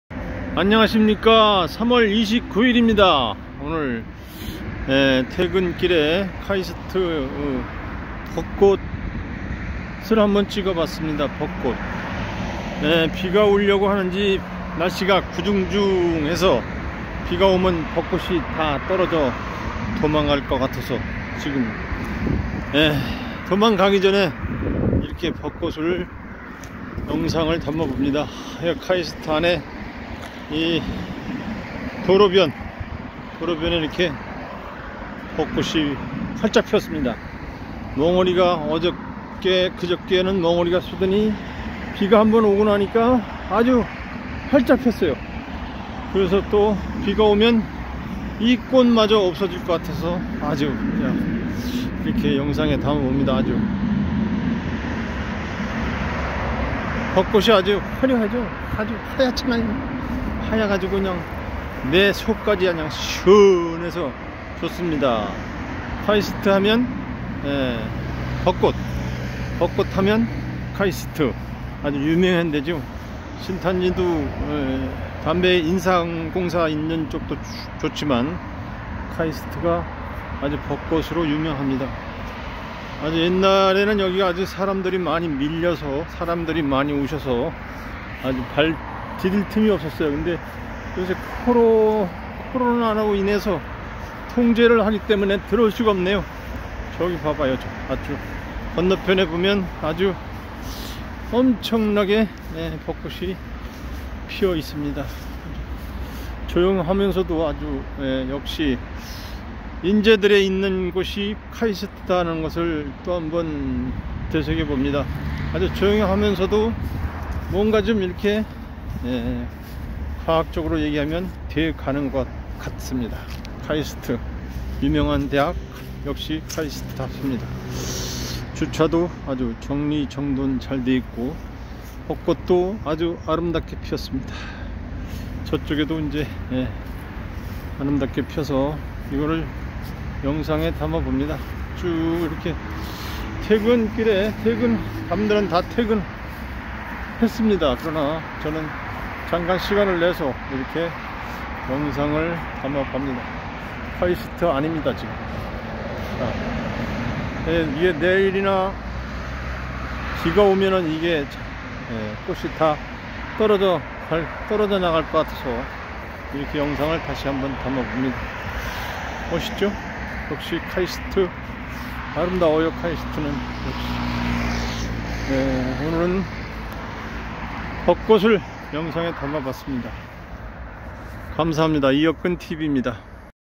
KAIST 내 벗꽂 아름답네요
퇴근길 카이스트 내에 비가 오기전에 벗꽃을 담어 보왔어요